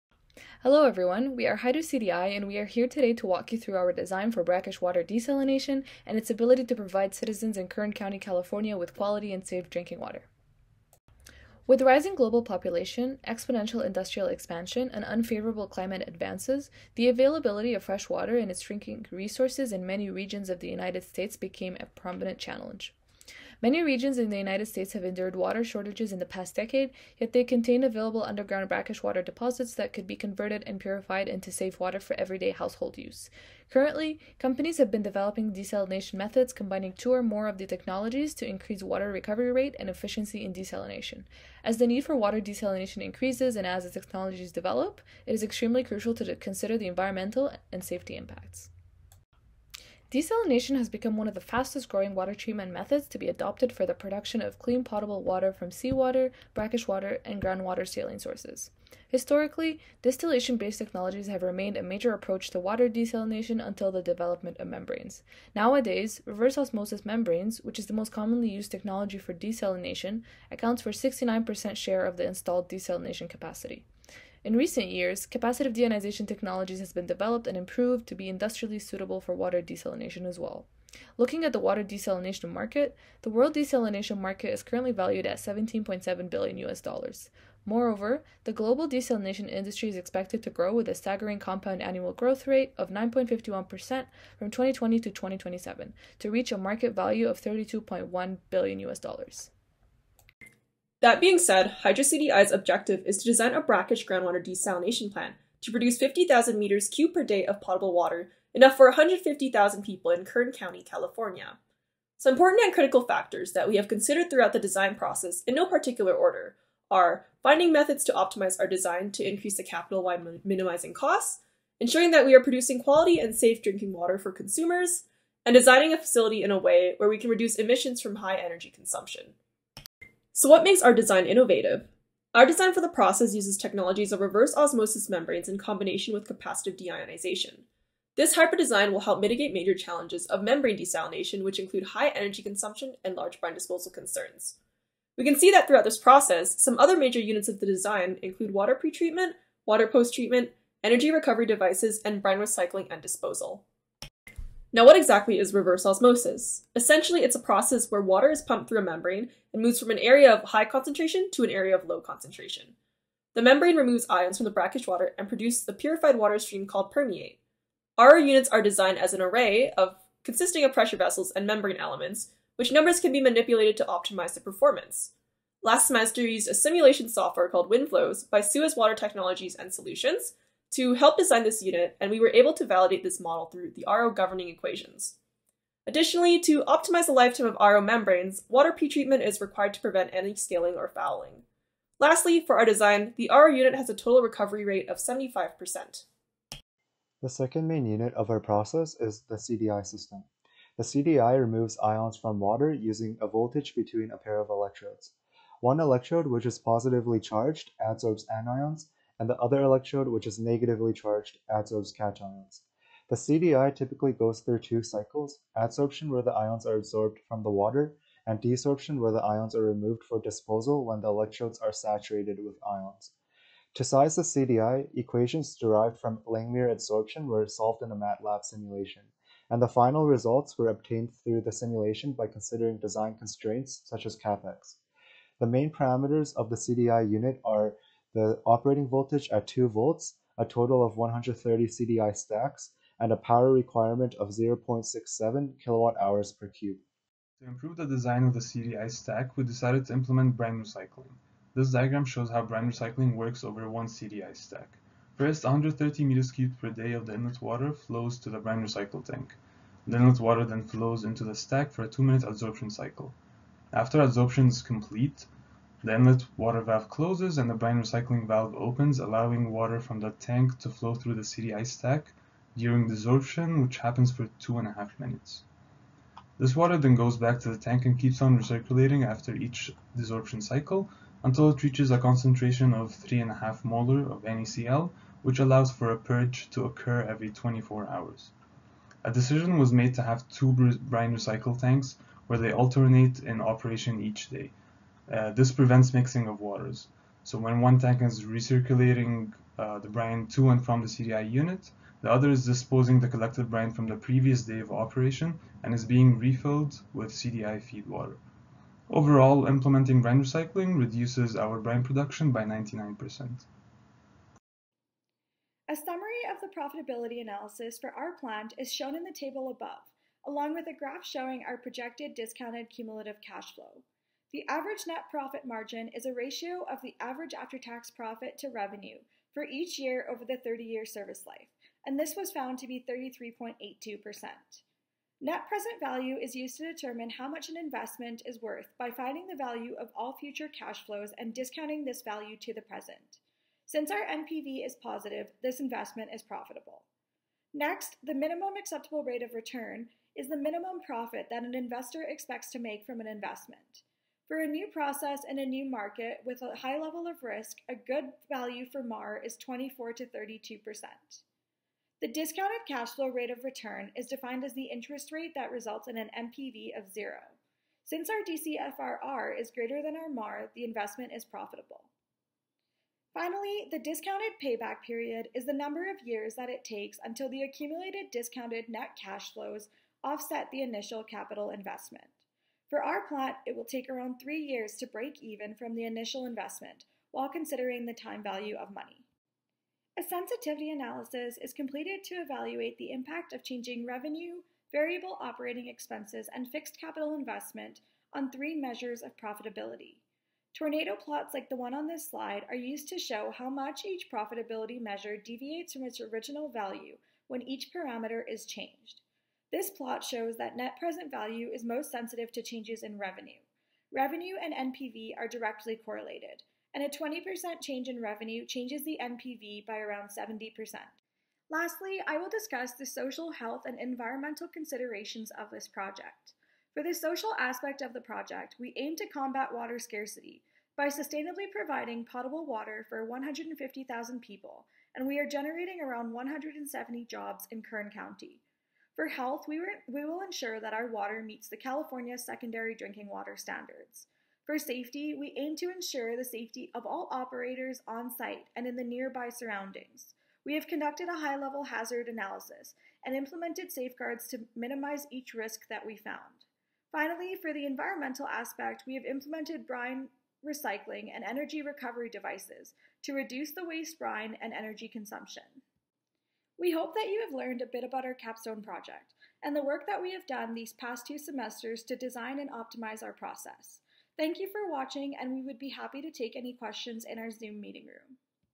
Desalination of Brackish Groundwater - Capstone Design Fair 2022
Desalination of Brackish Groundwater